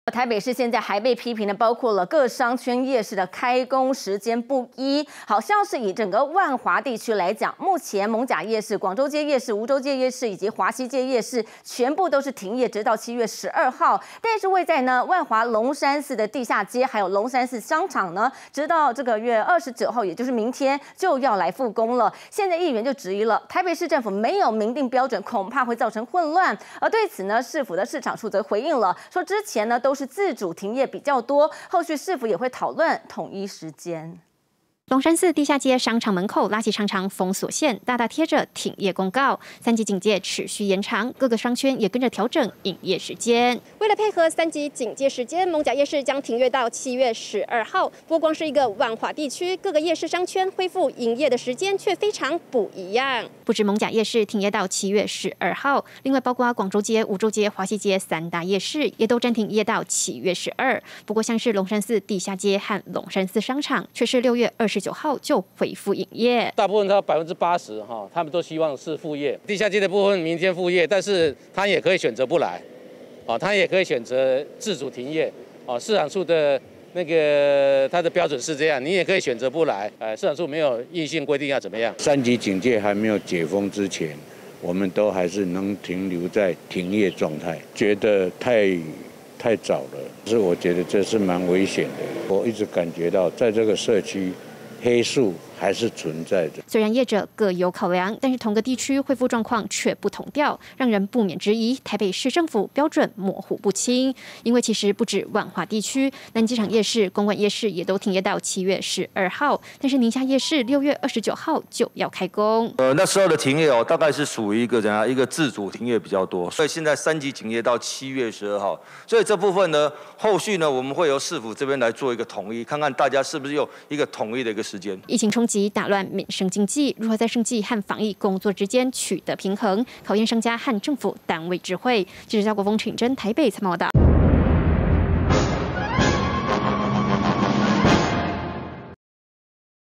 萬華區夜市明日復工！ 議員轟「復業標準不一」 北市府：將討論
三級警戒延長下，許多商圈都暫停營業，但是台北市卻出現各商圈夜市開工時間不一樣的狀況，像是艋舺夜市、廣州街夜市、梧州街夜市和華西街夜市，全都停業到7／12，但位在萬華的龍山寺地下街，還有龍山寺商場，卻在6／29就要復工。議員質疑，台北市政府沒有明訂標準，恐怕造成混亂，對此台北市政府市場處回應，之前都是自主停業較多，後續市府會討論統一時間。